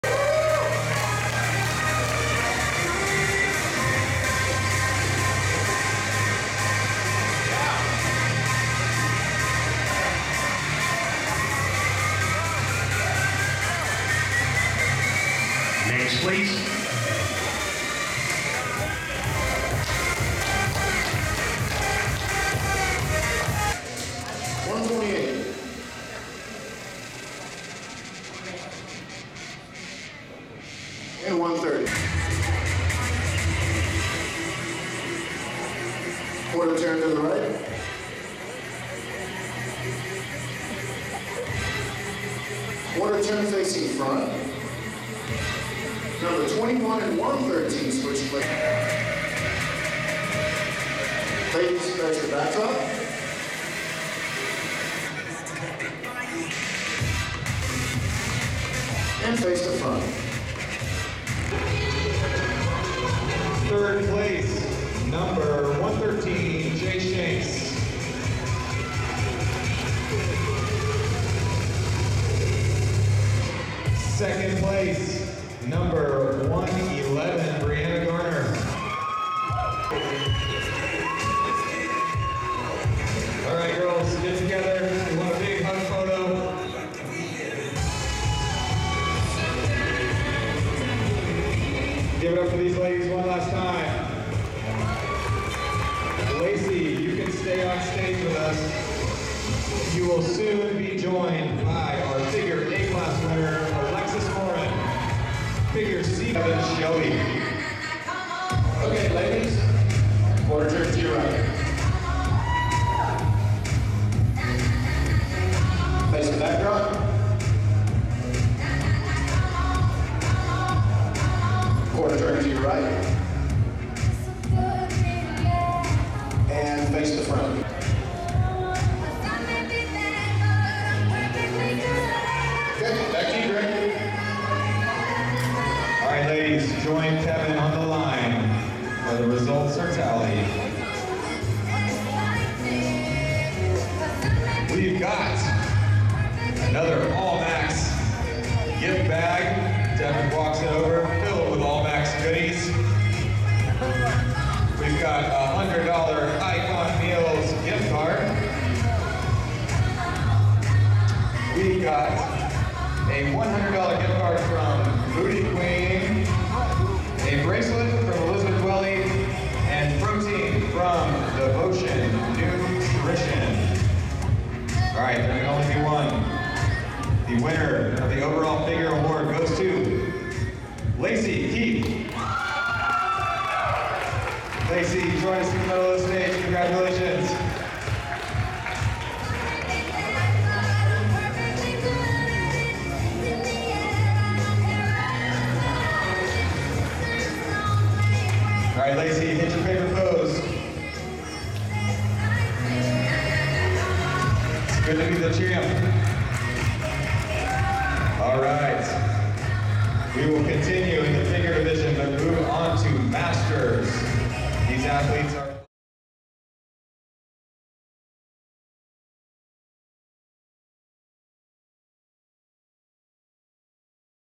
⭐️ NPC Figure Overall Winner Lacey Keath in 4K
Best Muscle Video showcases female athletes:
There are no pre-roll ads. 😃

Enjoy this new clip Best Muscle Video shot in 4K resolution of NPC Figure Competitor Overall Winner Lacey Winston Keath on stage.

Best Muscle Video only posts original content. 🎥